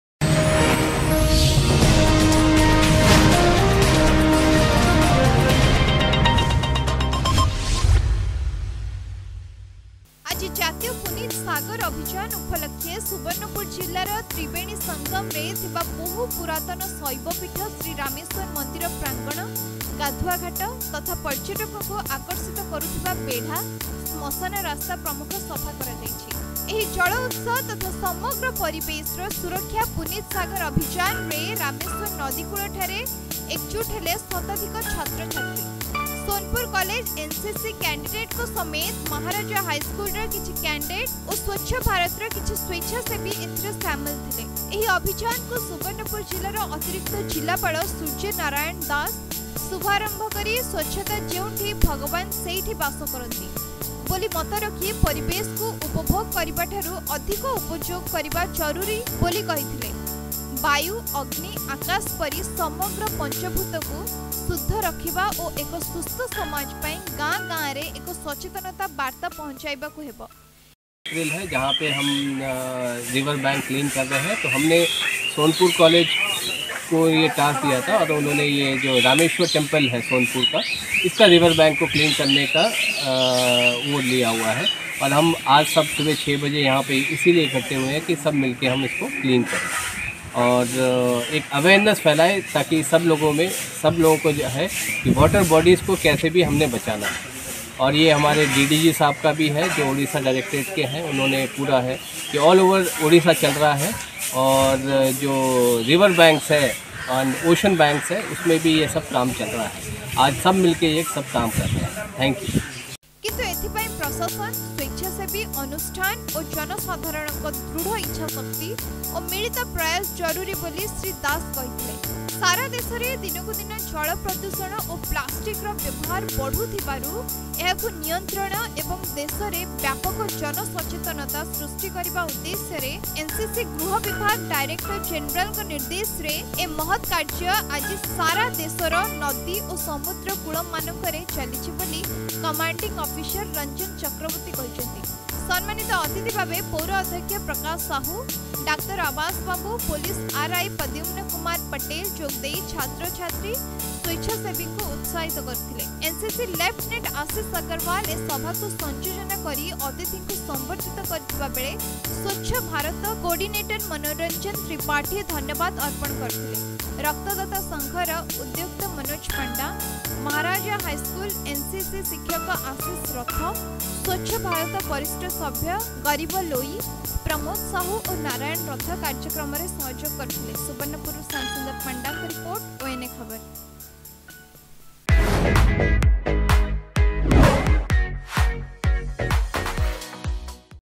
Sonpur College NCC students and volunteers join Puneet Sagar Maha Abhiyan
About Channel:
ଚ୍ୟାନେଲ୍ ବିଷୟରେ.